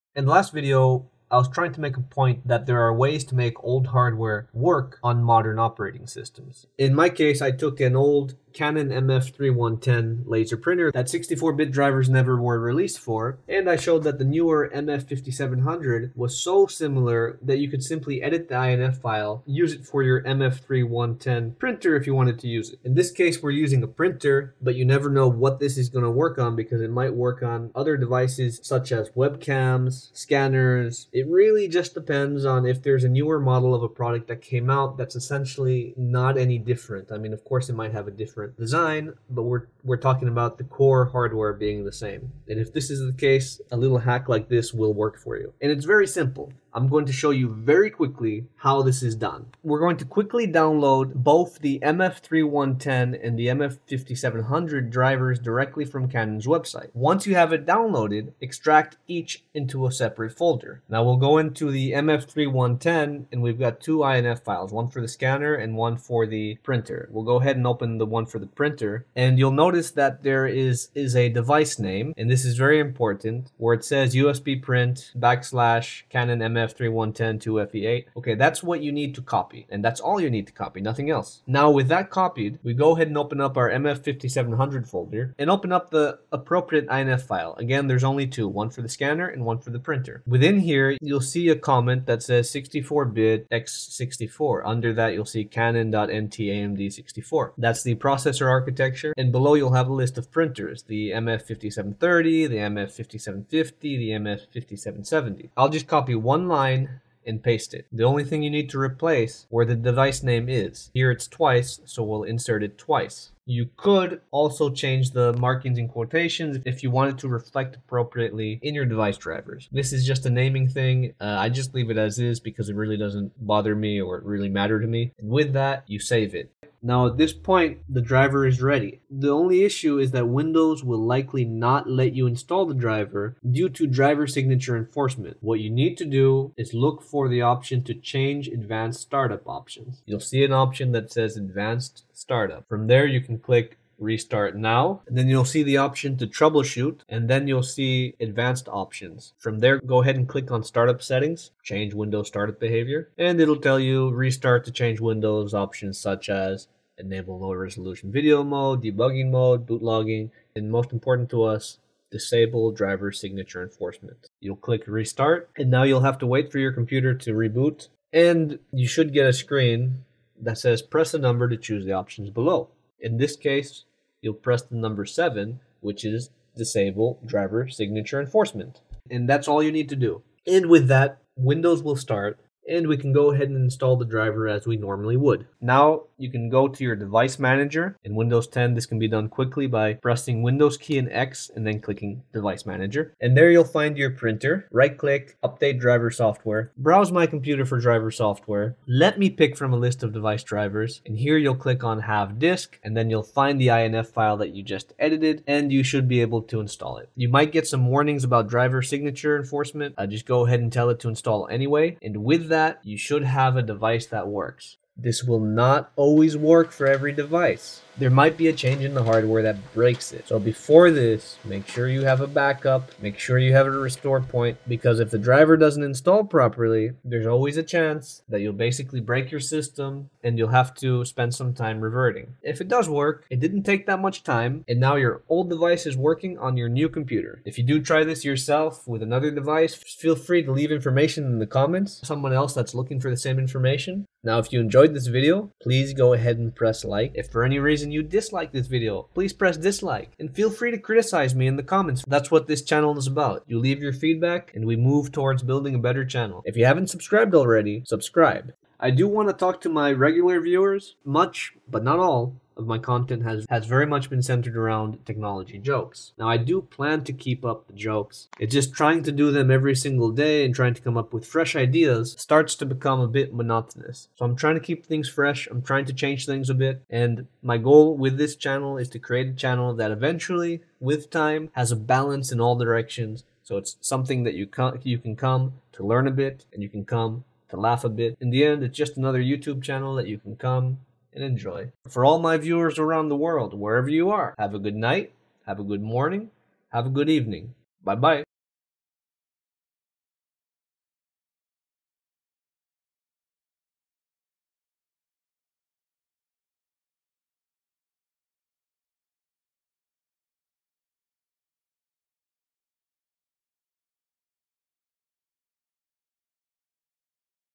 Modify .inf file to make old hardware work (MF3110 example)
This is an example of how to modify an inf file to attempt to make old hardware run on windows 10